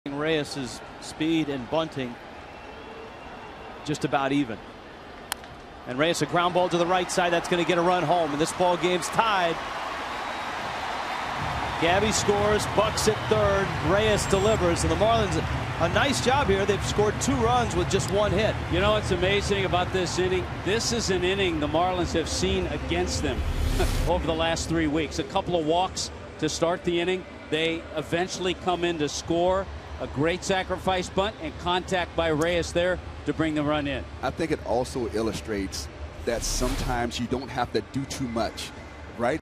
2012/06/27 Reyes' RBI groundout
STL@MIA: Reyes ties the game with an RBI groundout